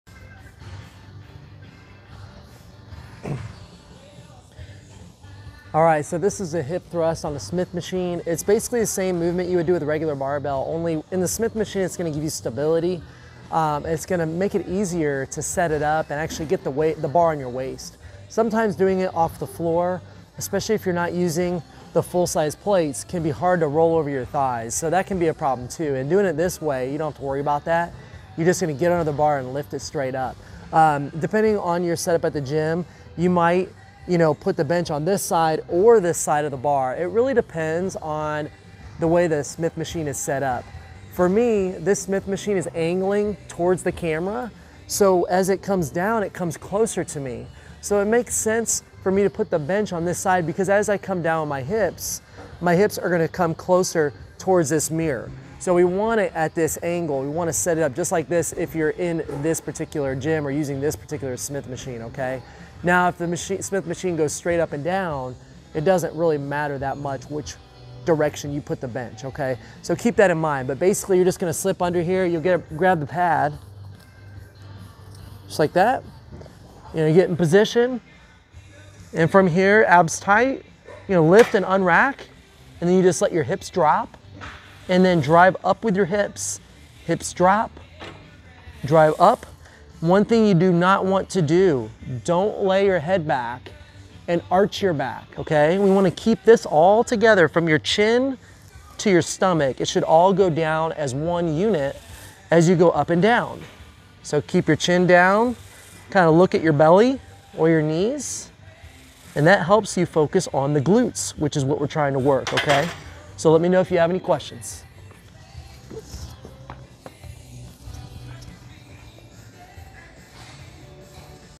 How to: Smith Hip Thrust
This is a great hip thrust variation to use if you hate setting up a regular barbell!

Have you tried it before?